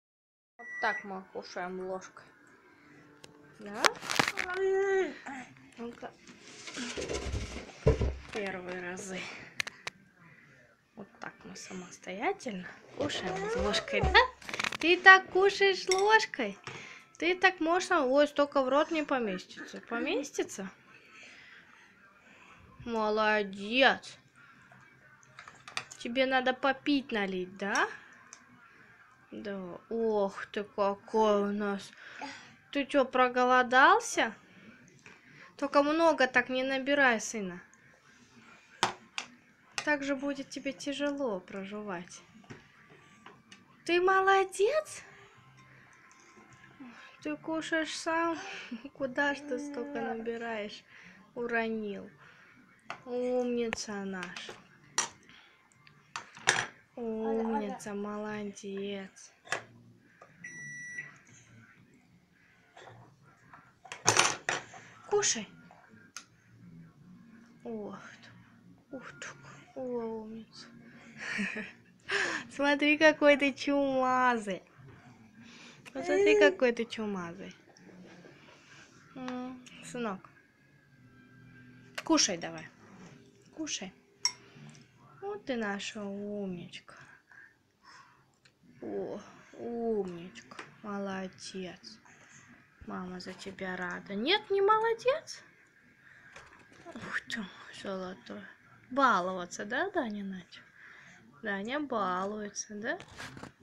Аутизм - излечим! (C) Центр "БЕЭР ДАВИД"( Иерусалим) Освоение ложки, как результат сеансов
Аутизм - излечим! (C) Центр "БЕЭР ДАВИД"( Иерусалим) Освоение ложки, как результат дистанционного лечения в Центре